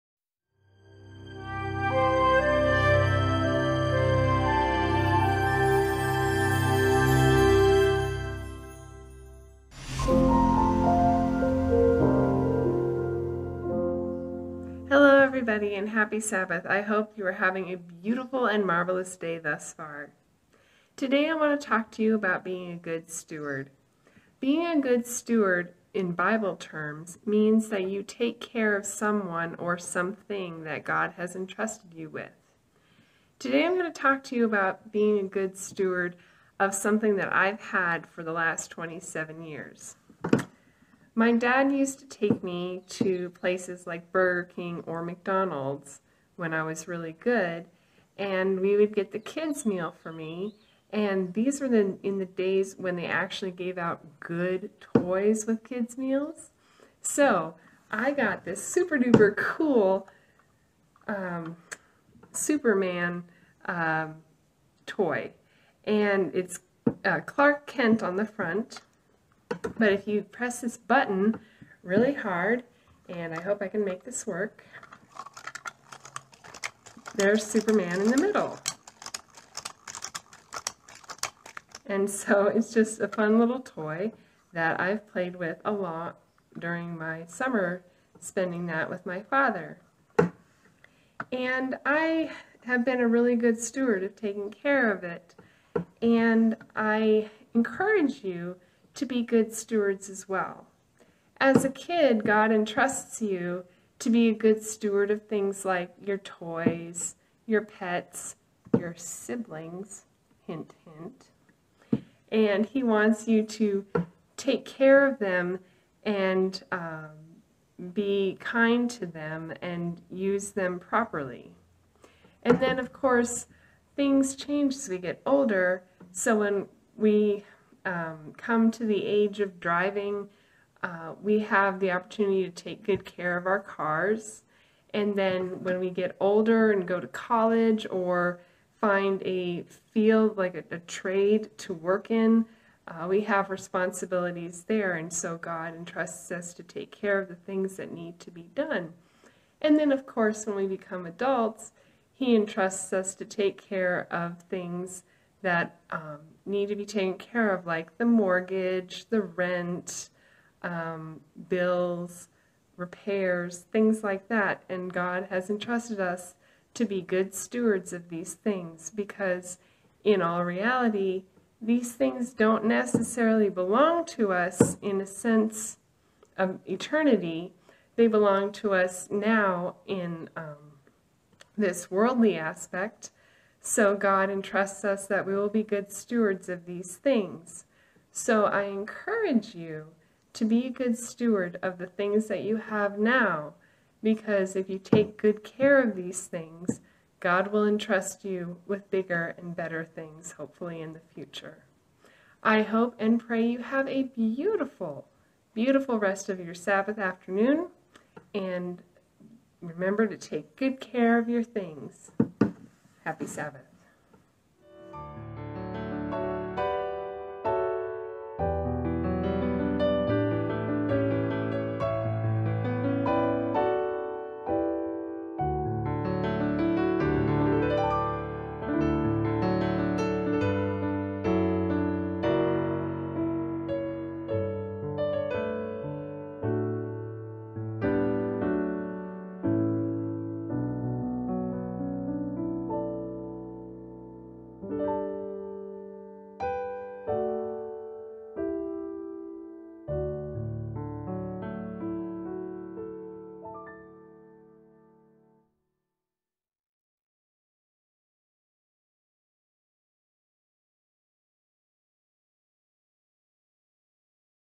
Children's Story by Valerie Ray
Valerie Ray talks about stewardship of the things we have been given.